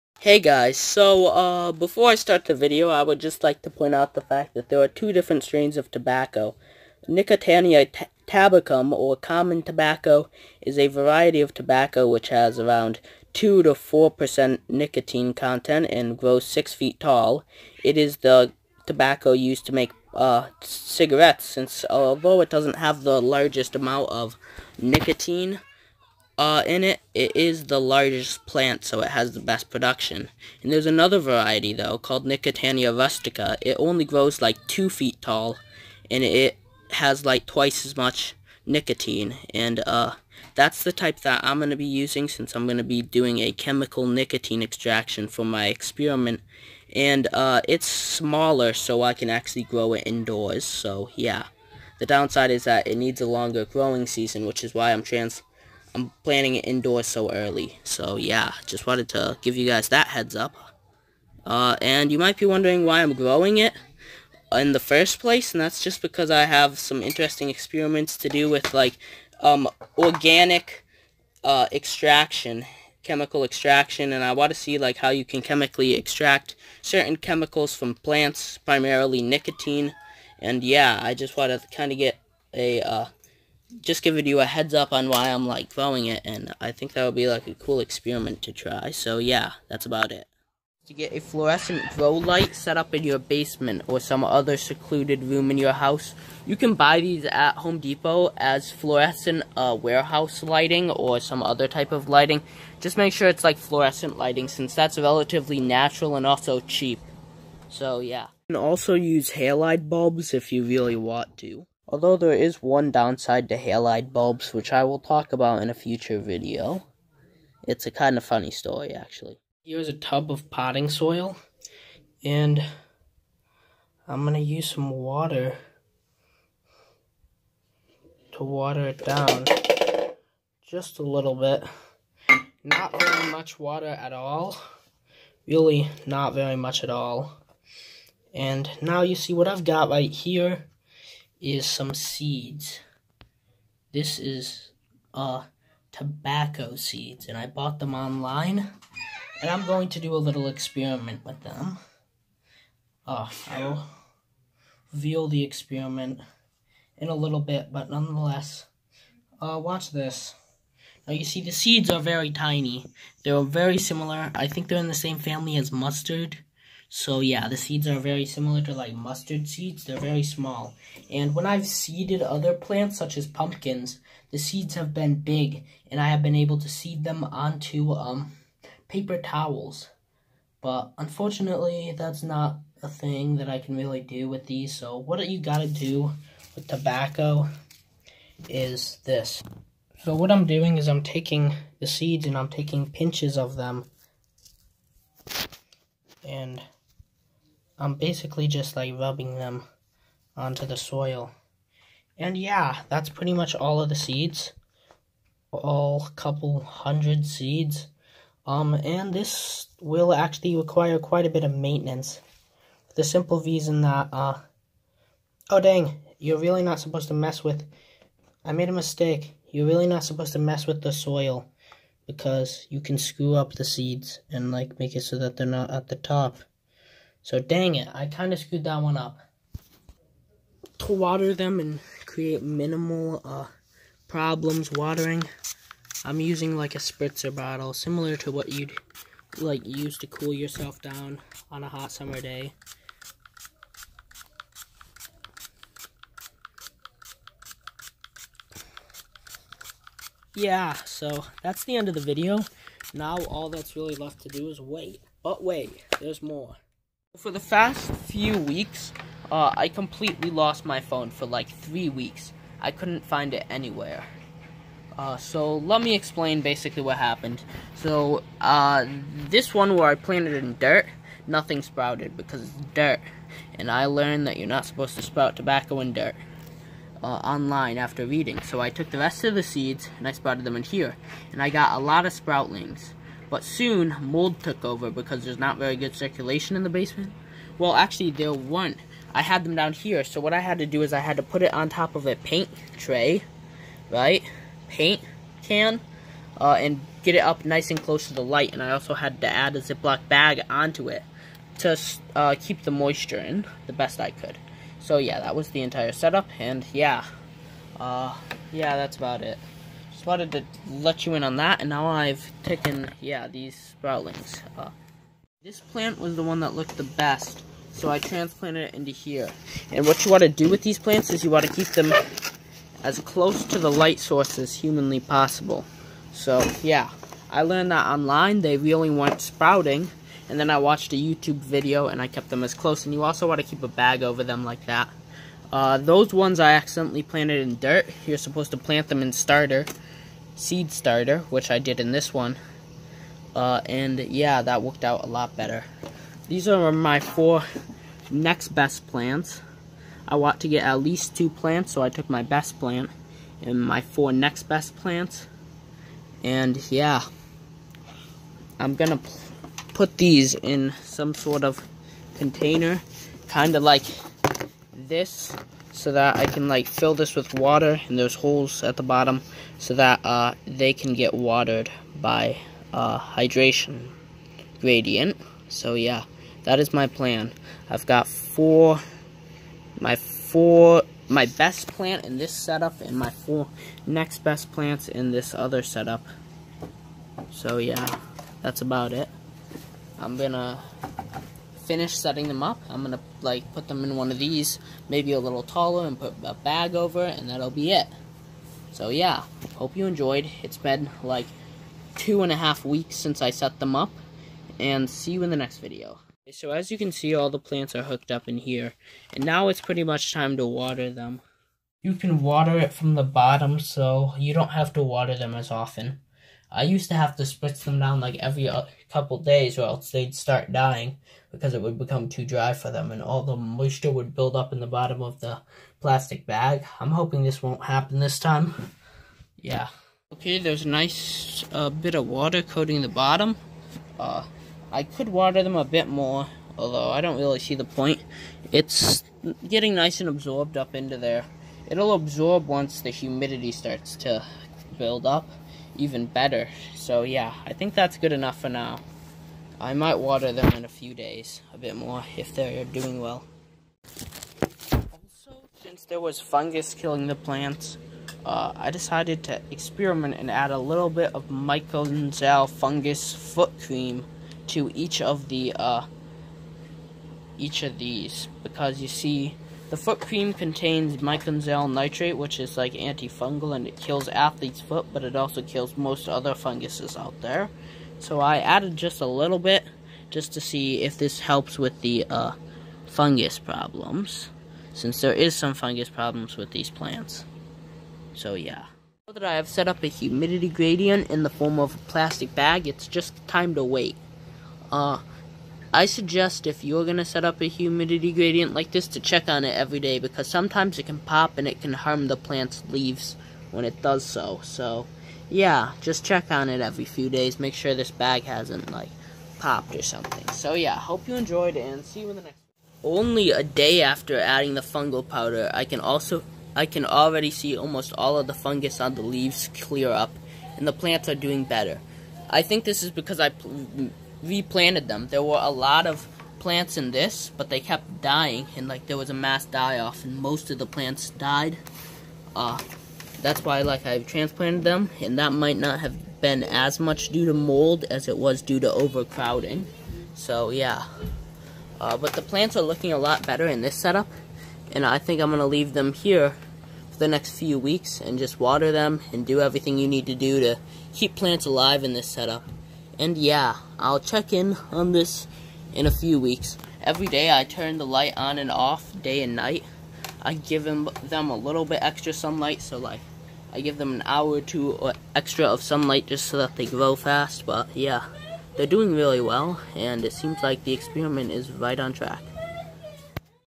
Growing nicotiana rustica (tobacco) indoors episode 1
I want to do experiments on organic extraction and what better way to start than with nicotine from tobacco?